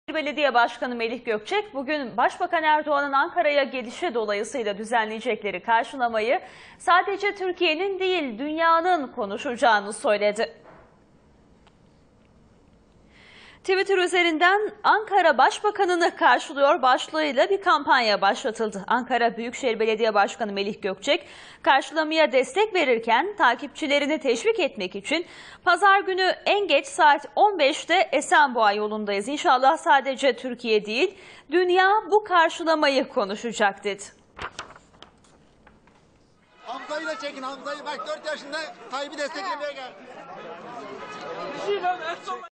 Ankara'da Erdoğan'ı karşılama hazırlığı
Başbakan Recep Tayip Erdoğan'ın Kuzey Afrika dönüşü İstanbul'da coşkulu bir kalabalık tarafından kaşılanmasının ardından Ankara da Başbakan için hazırlık yapıyor